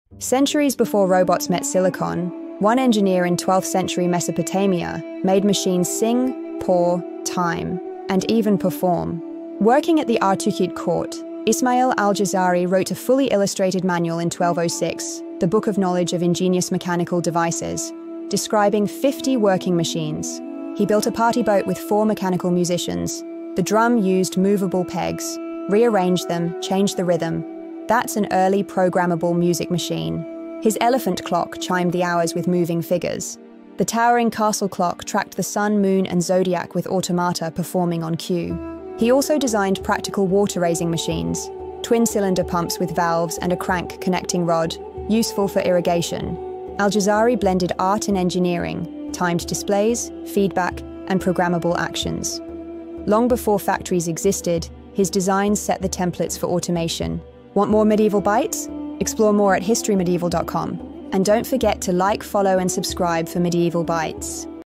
“Al-Jazari: The Medieval Master of Machines”
Ismail al-Jazari (1136–1206) turned water, weights, and clever gearing into musical automata, programmable rhythms, and monumental clocks. His 1206 illustrated treatise documents 50 working devices—from the Elephant Clock to twin-cylinder pumps—laying foundations for automation in the medieval Islamic world.